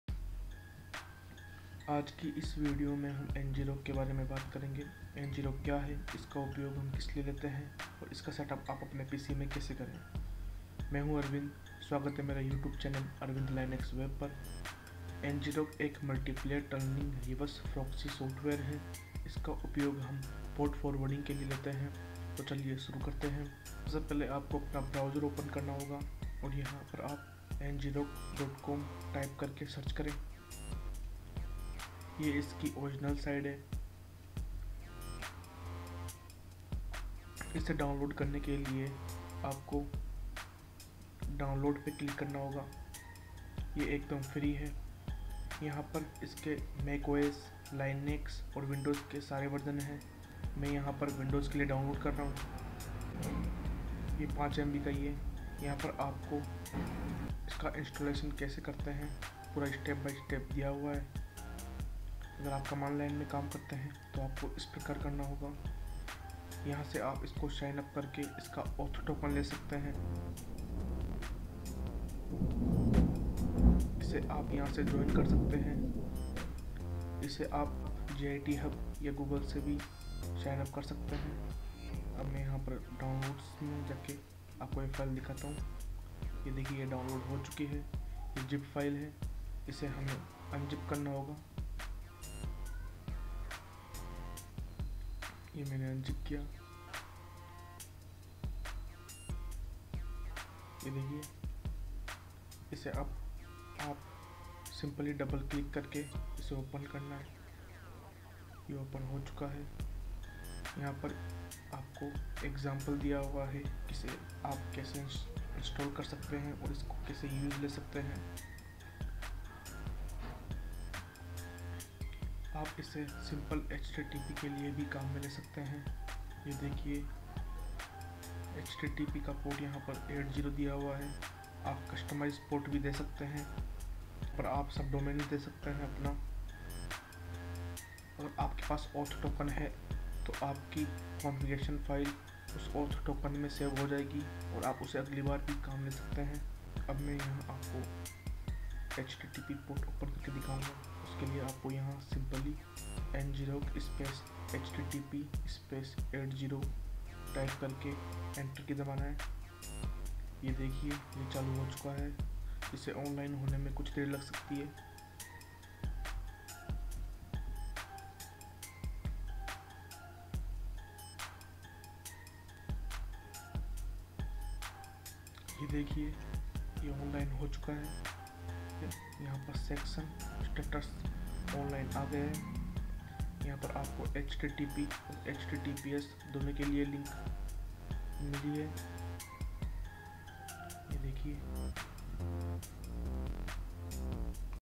How To Use Ngrok For Port Forwarding In Windows 10
These videos are about ngrok tools.This tool is used to make ports forward. Ngrok is a multiplatform tunneling, reverse proxy software
ngrok creates a tunnel from the public internet to a port on your local machine. You can give this URL to anyone to allow them to try out a web site you're developing without doing any deployment.

MY BLOGS :--